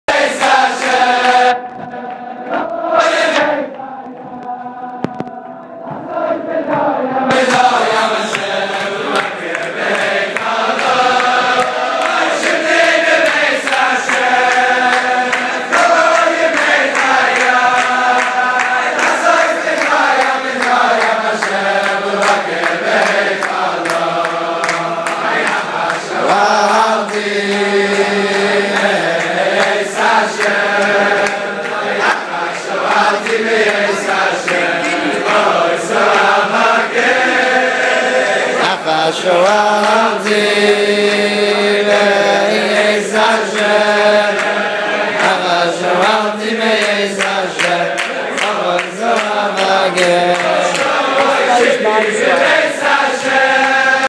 YU Beis Medrash Dancing with Rav Moshe Weinberger 2012
After inspiring words of Torah from Rav Moshe Weinberger in YU. September 2012. Elul 5772.